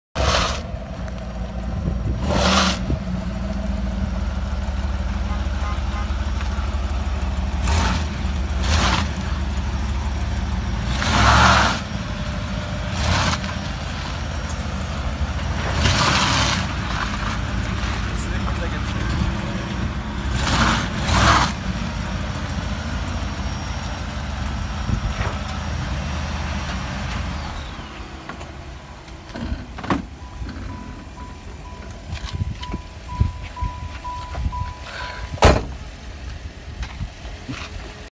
Mini Burnout in Reverse lol CAMARO at Auction
Part 2 of the first video!
Doing a burnout in reverse! Lol